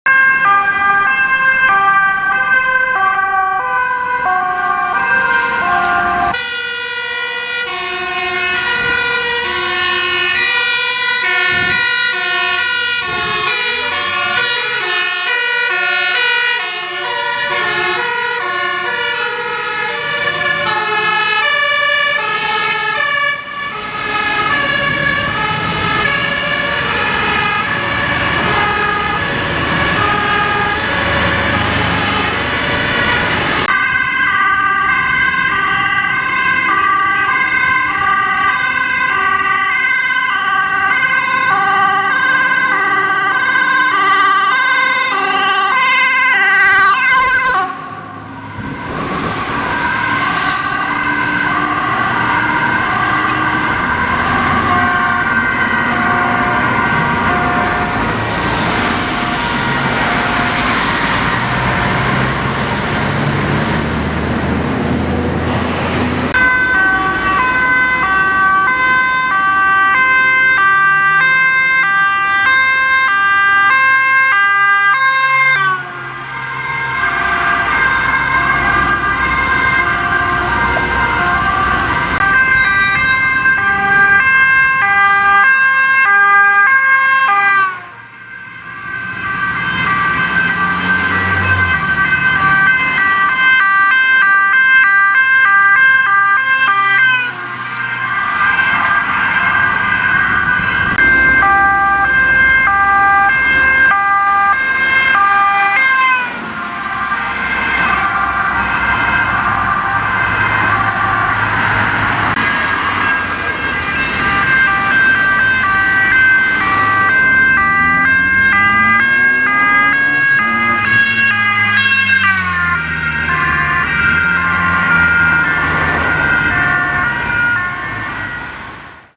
Brandweer en politie rukken groots uit naar een brand in een Winkelcentrum in Purmerend
Brandweer en politie rukken groots uit naar een brand in het Willem Eggertcentrum Winkelcentrum in Purmerend. De politie reed met veel eenheden naar de brand om de in/uitgangen van het winkelcentrum af te sluiten.

- Clips recorded using a Panasonic HC-V770
- Videos edited & compiled using Magix Deluxe Premium
Ⓒ 112NoordWestNL